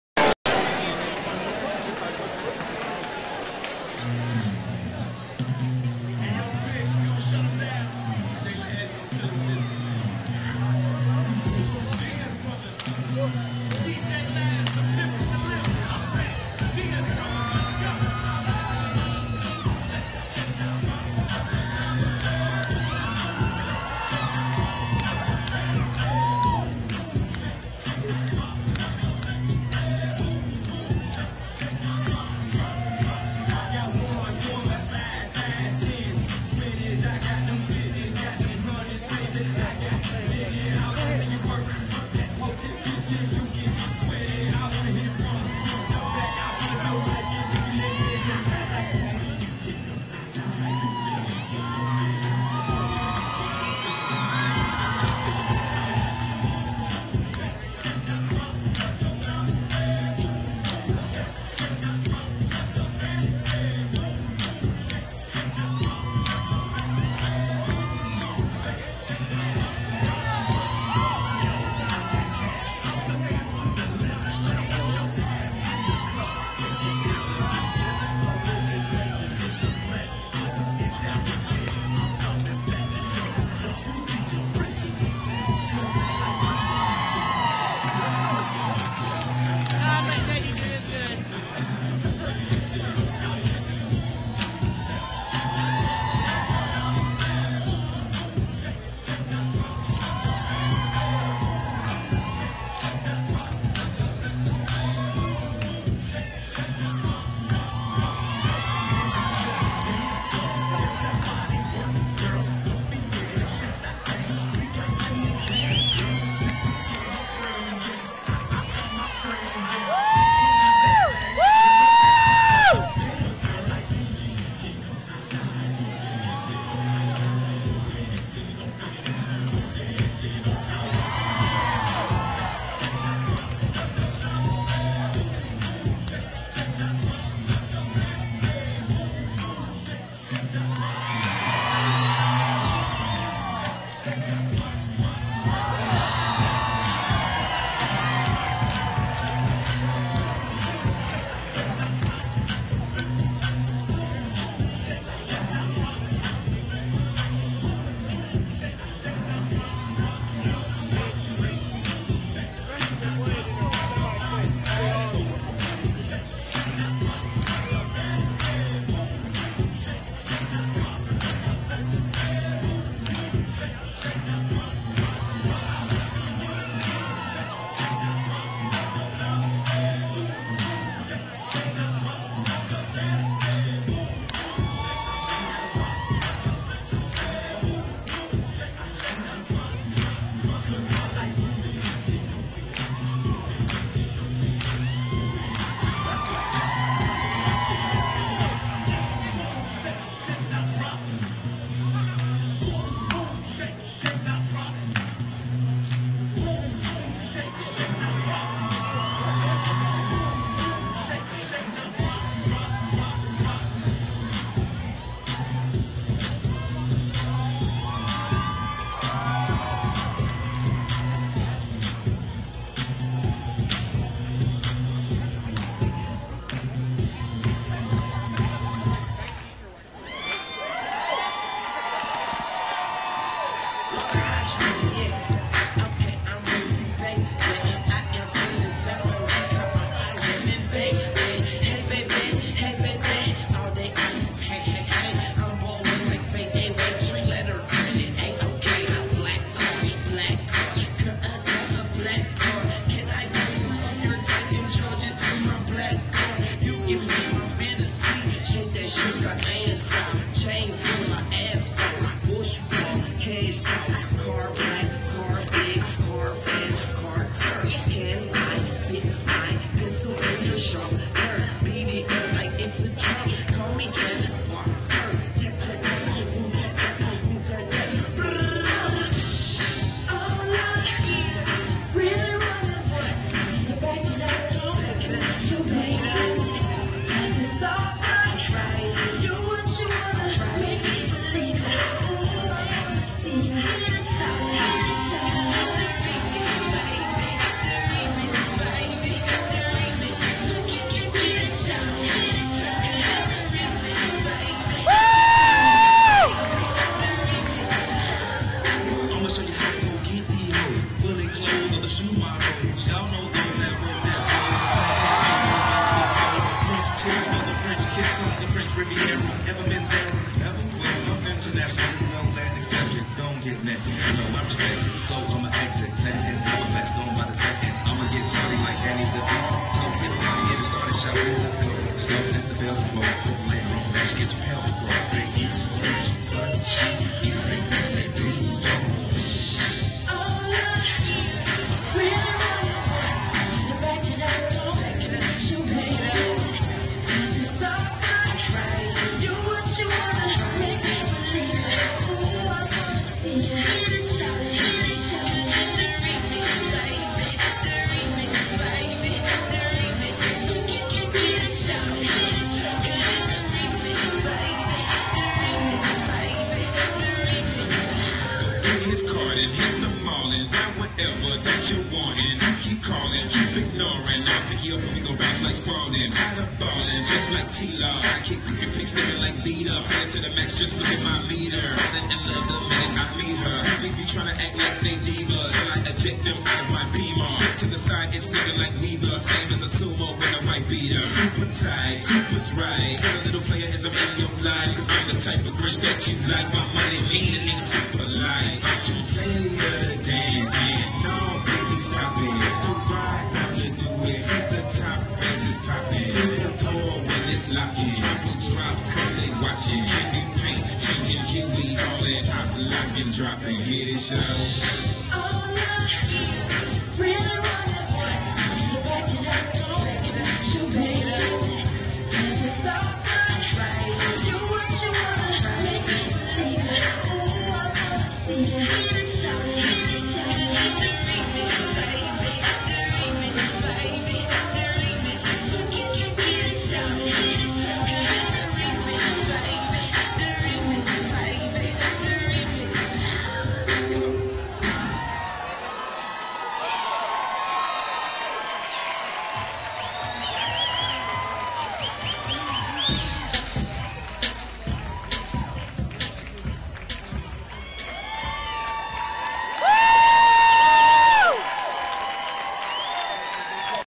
JayDee & Crew Camera 2nd half-time show @ Roller Derby-Columbia, SC 1-23-2010
JayDee & his crew did the half-time show @ the Roller Derby in Columbia, SC on January 23, 2010
Columbia Quad Squad VS Low Country High Rollers B Teams